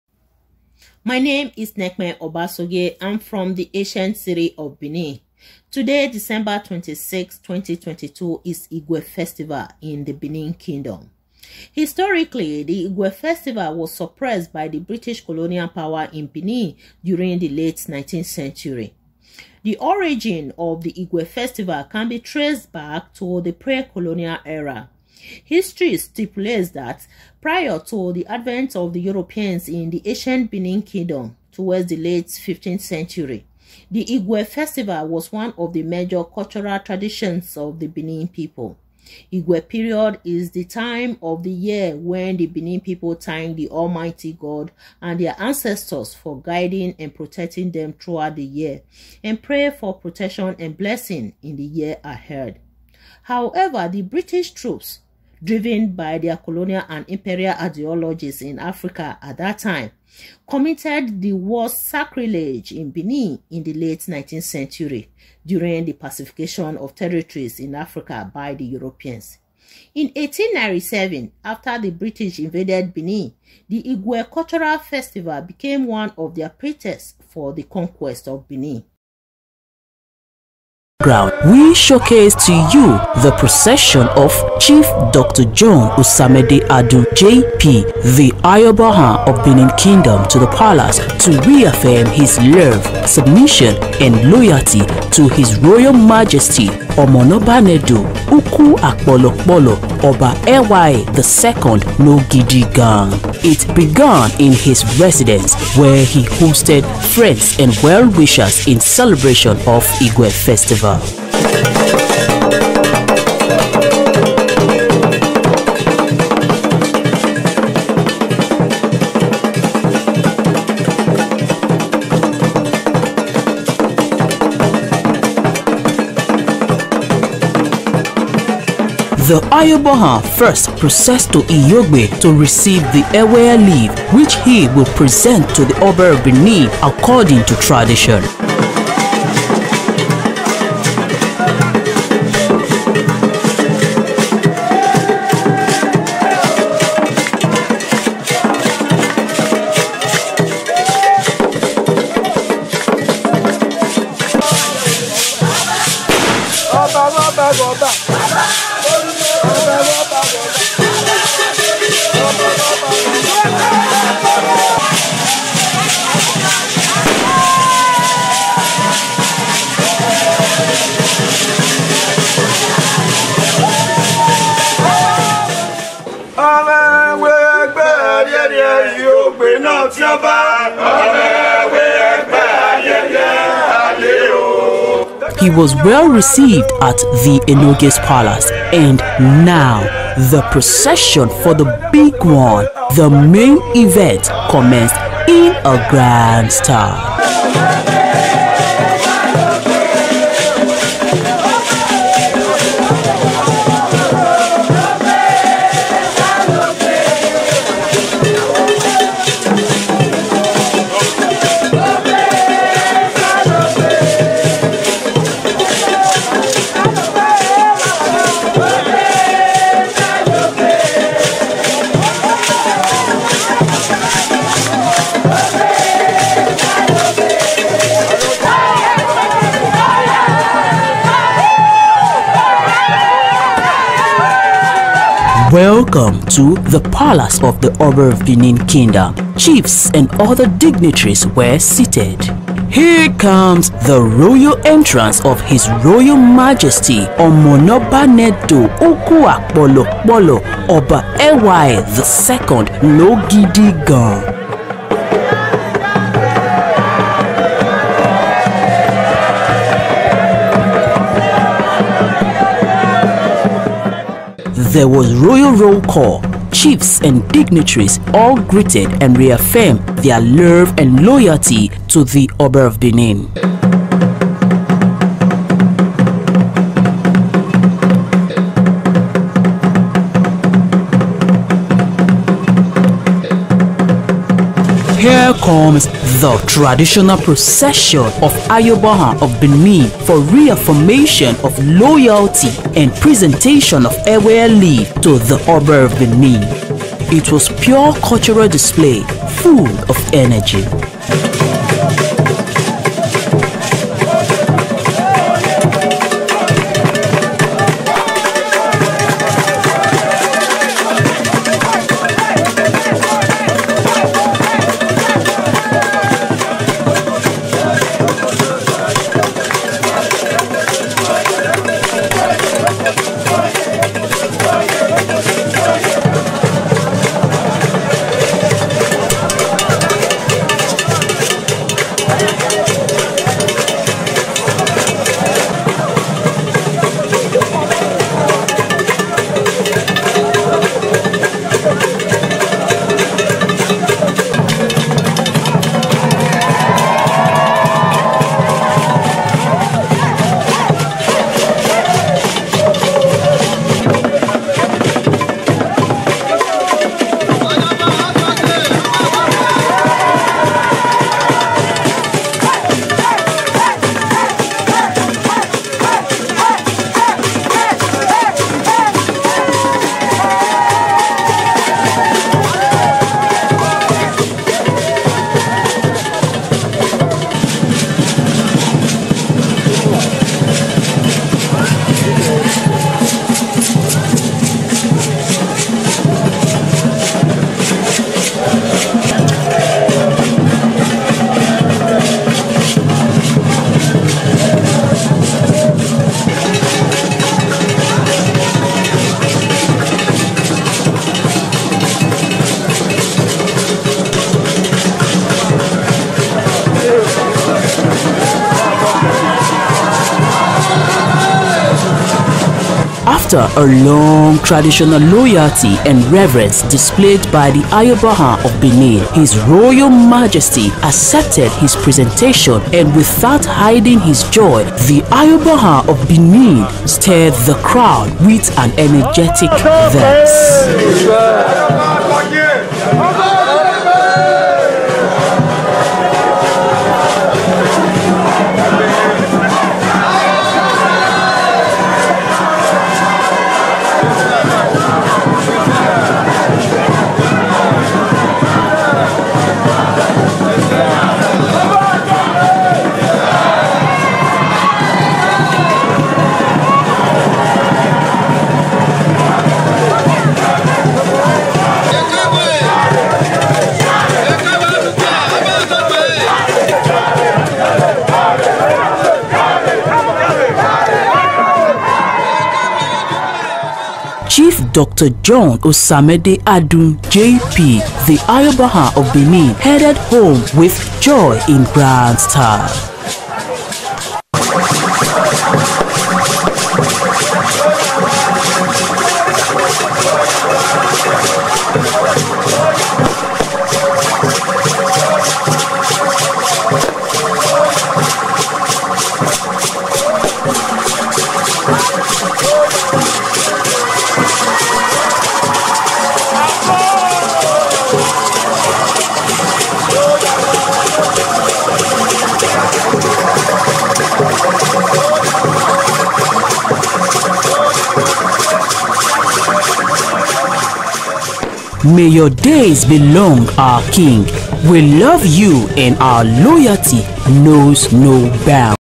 The British Suppressed Igue Festival In Benin in 1897
Igue Festival in the Benin Kingdom was Suppressed by the British Colonial Power